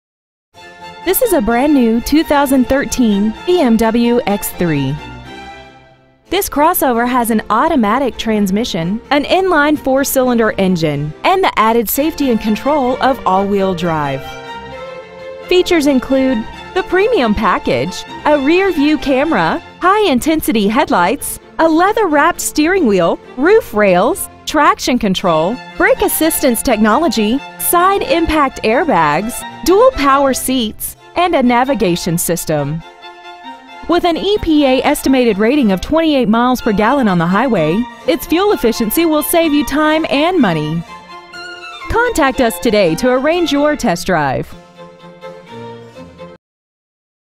2013 BMW X3 Monrovia CA 91016
Year : 2013
 Make : BMW
 Model : X3
 Engine : 2.0L I-4 cyl
 Trans . : Automatic
 Exterior : Space Gray Metallic
 Miles : 1
 Interior : Black
 BMW of Monrovia
 2013 BMW X3 Monrovia CA